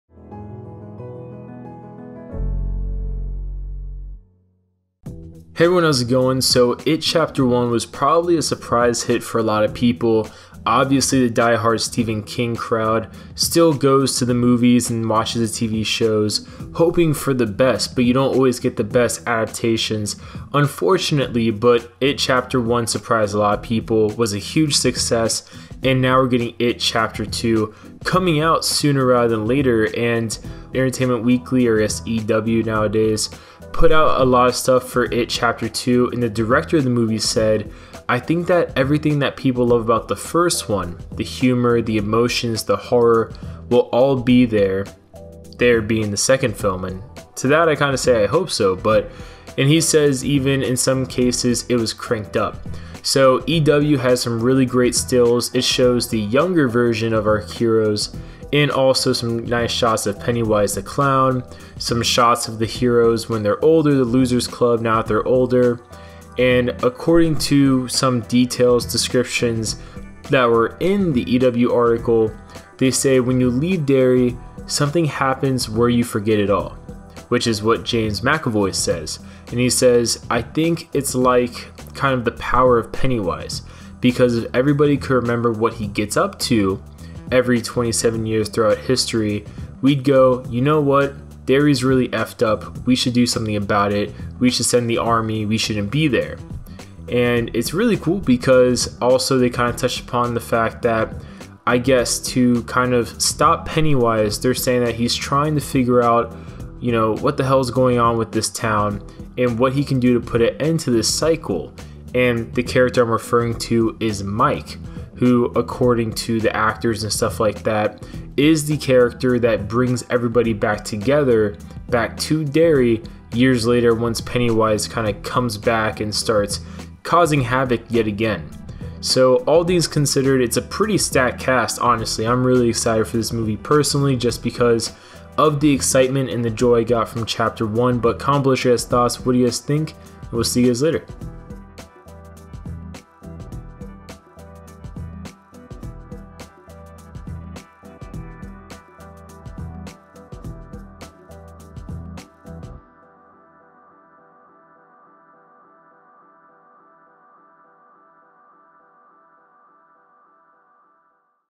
New IT Chapter 2 Details and Images Reveal Adult Losers Club
Entertainment Weekly has revealed their exclusive Comic-Con issue that highlights IT Chapter 2, with connecting covers featuring Pennywise and the Losers Club.